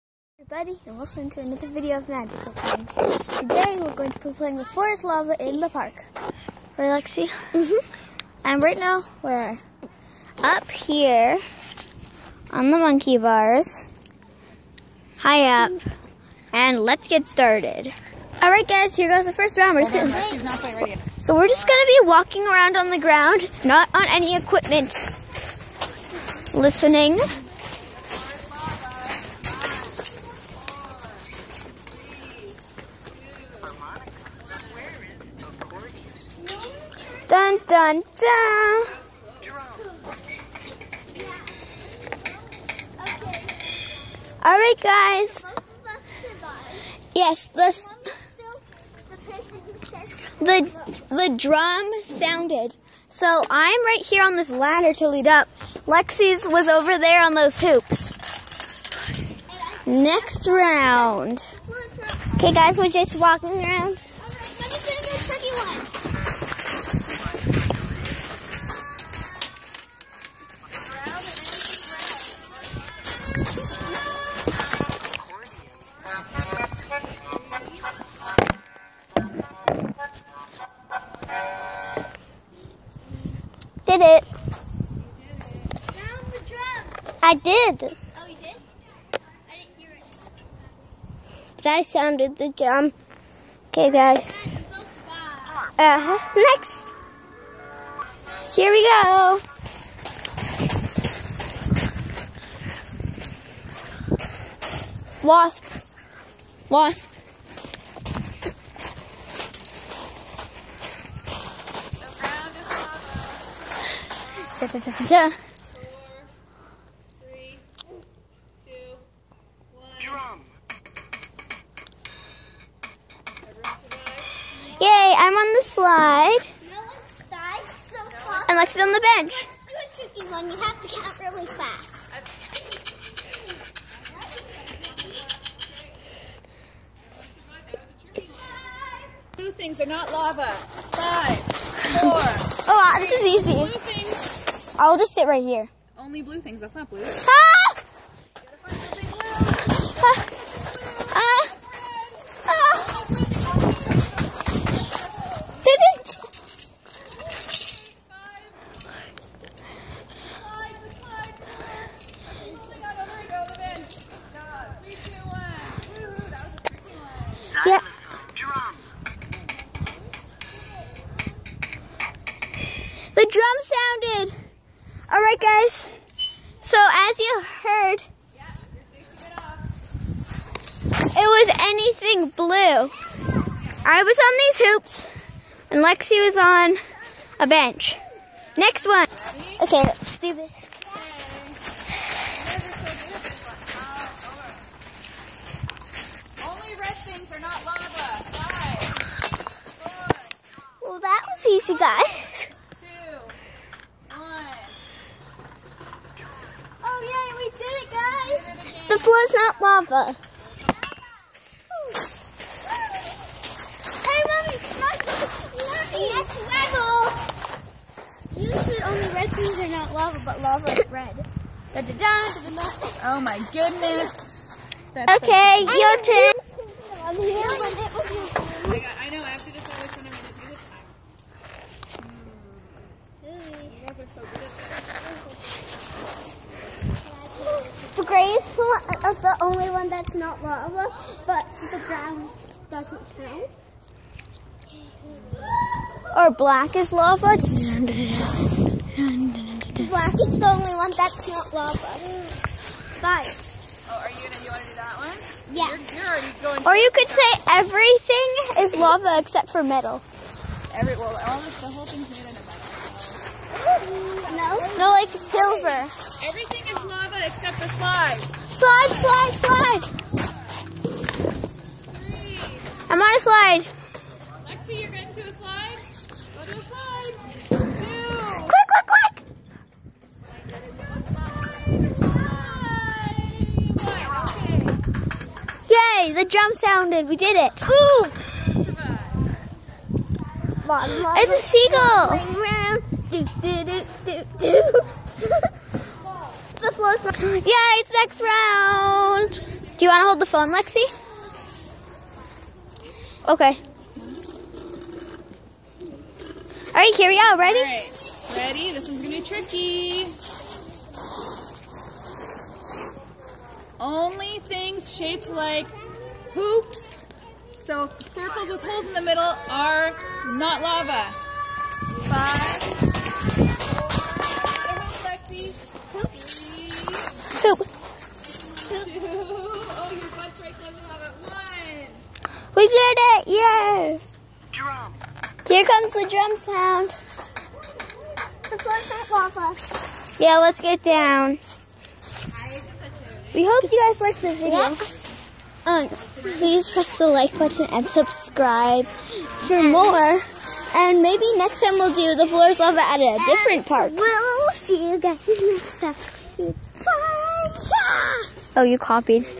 The Ground Is Lava!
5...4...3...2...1... the ground is lava at a park!